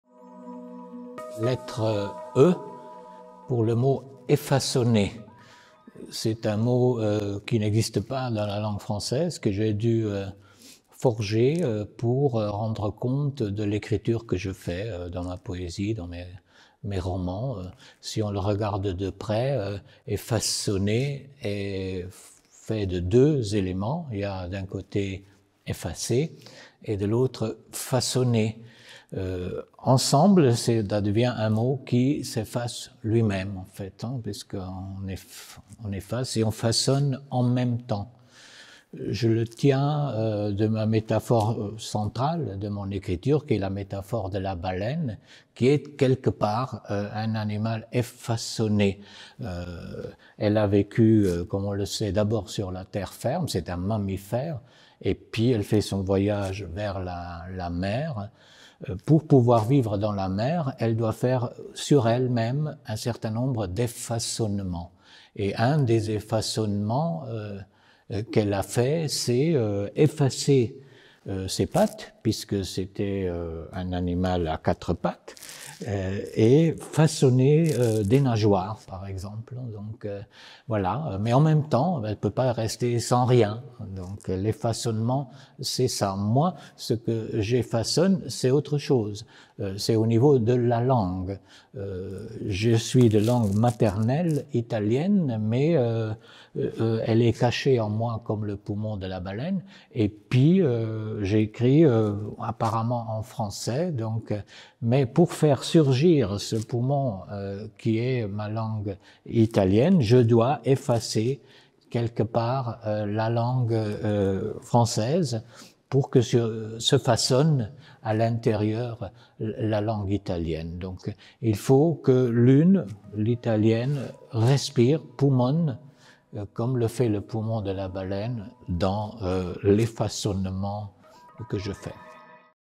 Si vous inventiez un mot, lequel serait-ce ?
***Jean Portante – E comme Effaçonner ***
En résonance avec l’exposition "Freigeister", (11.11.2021 – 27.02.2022) le Mudam a invité vingt-sept personnes d’horizons variés à définir vingt-six mots pour former un abécédaire vidéo allant de « ailleurs » à « zesummen ». Ensemble, ces courtes interventions filmées esquissent un portrait subjectif et fragmentaire de la société luxembourgeoise et portent un regard sur les problématiques qui traversent le monde contemporain.

À voir au Mudam et en ligne